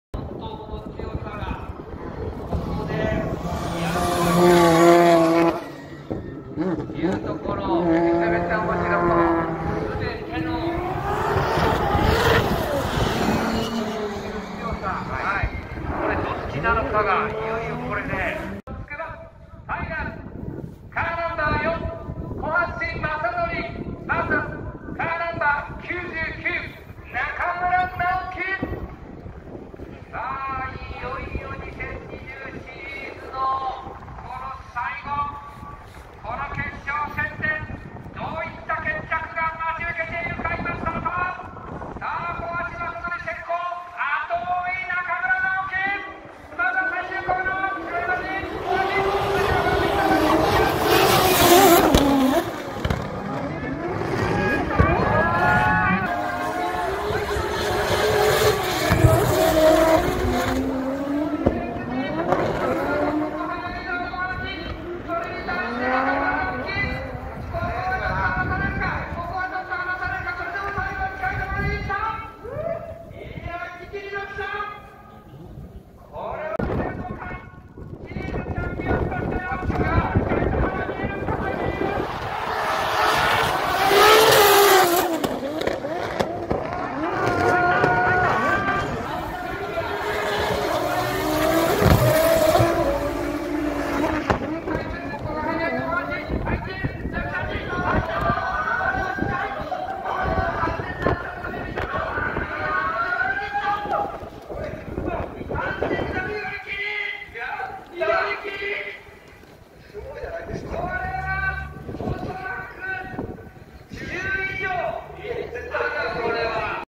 D1GP Rd 8 TSUKUBA 決勝 筑波サーキット 追走
2020 D1GP Rd 8 TSUKUBA 決勝 筑波サーキット 追走 (2021/01/31)
Drift Final S15 Silvia
Kohashi Masanori
           VS
Nakamura Naoki